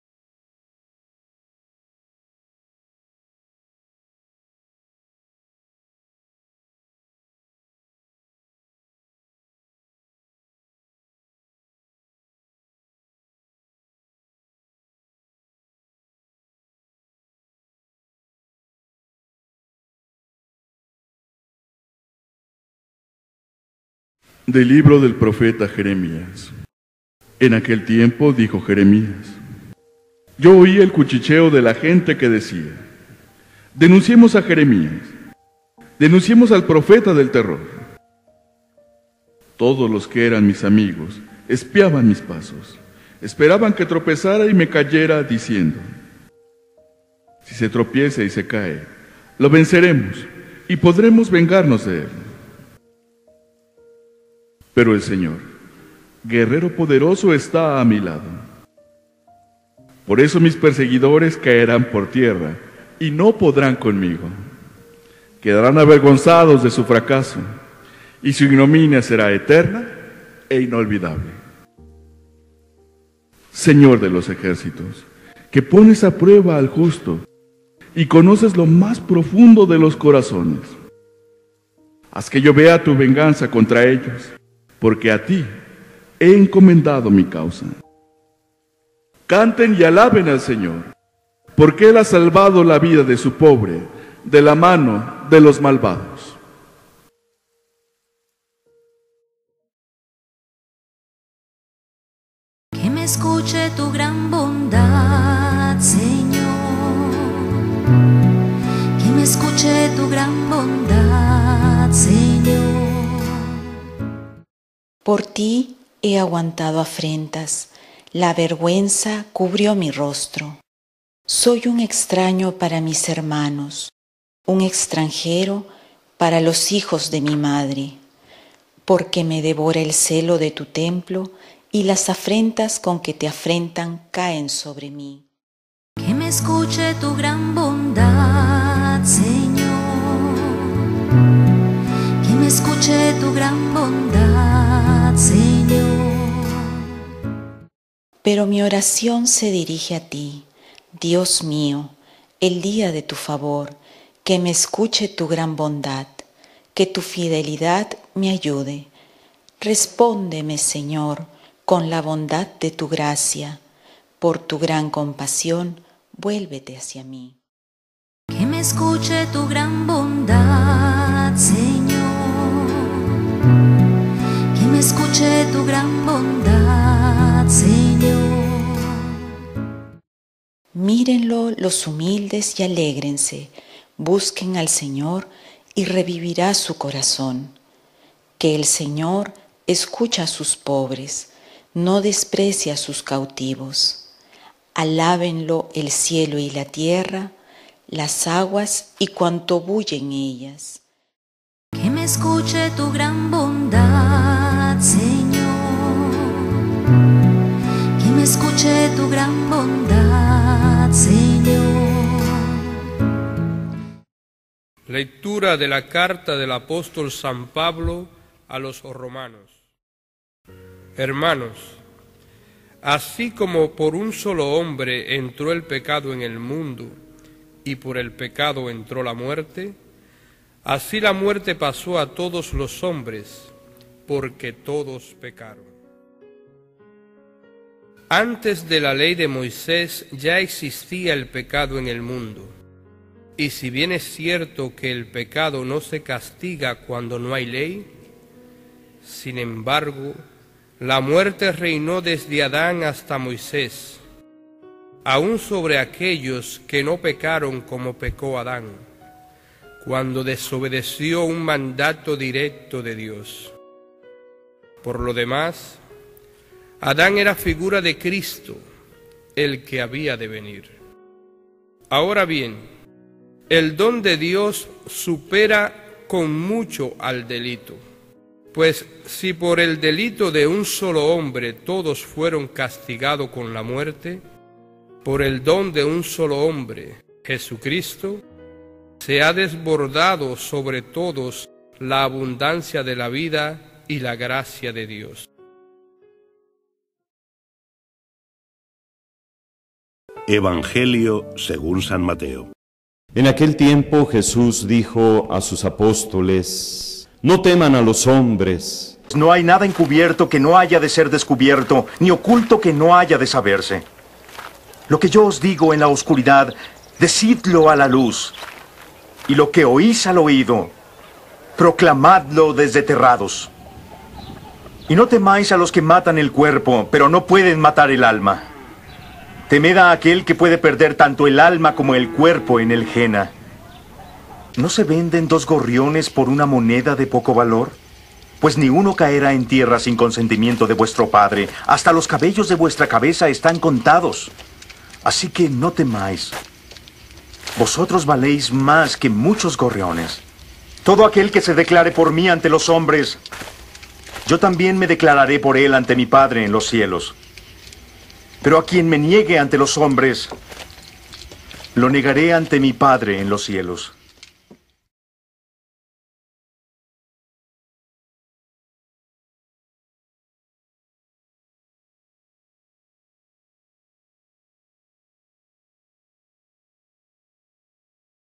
EVANGELIO Y LECTURAS DOMINGO 25 DE JUNIO DE 2023 | DOMINGO DUODÉCIMO T. ORDINARIO | MATEO 10, 26-33
EVANGELIO Y LECTURAS EUCARISTÍA DOMINGO 25 DE JUNIO DE 2023 DOMINGO XII DUODÉCIMA SEMANA DEL TIEMPO ORDINARIO CICLO A.  Primera Lectura: Del libro del profeta Jeremías 20,10-13 SALMO 68,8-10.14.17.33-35 R/. Que me escuche tu gran bondad, Señor. Segunda Lectura: De la carta del apóstol san Pablo a los Romanos 5,12-15 Evangelio según San Mateo 10,26-33